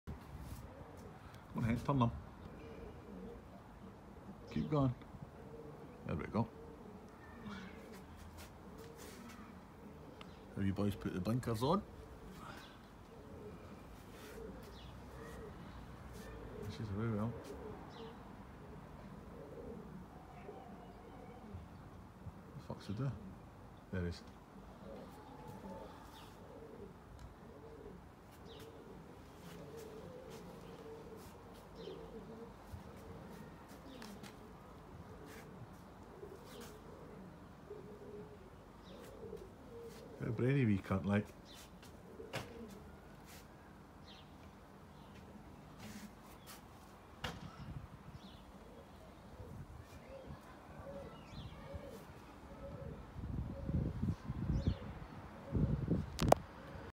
Tackling Heepsy (blue doo)
Tacking a wee blue doo that’s got over 500 hens to his name